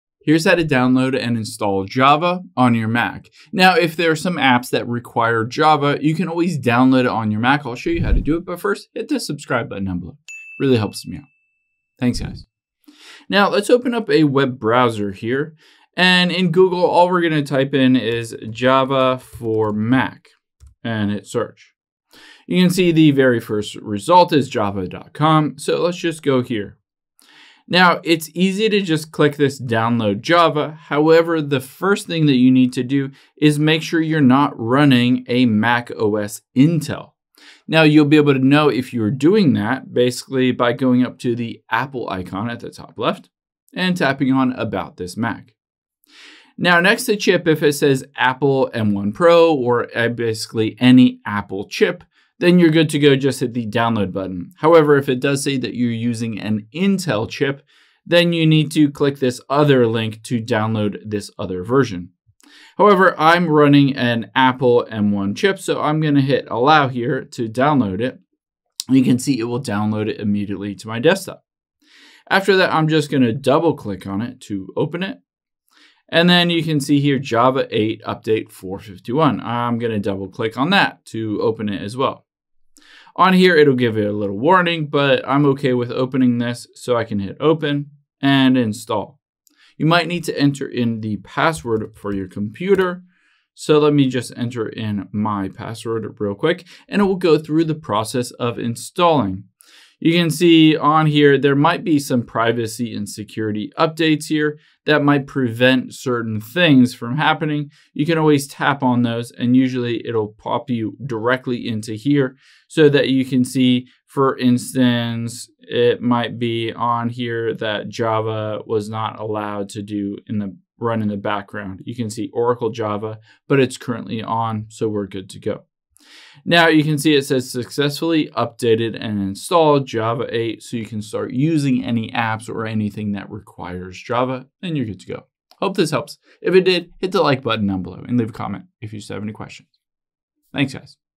How To Download And Install Java On Mac
Here's how to download and install the latest Java on your MacBook computer.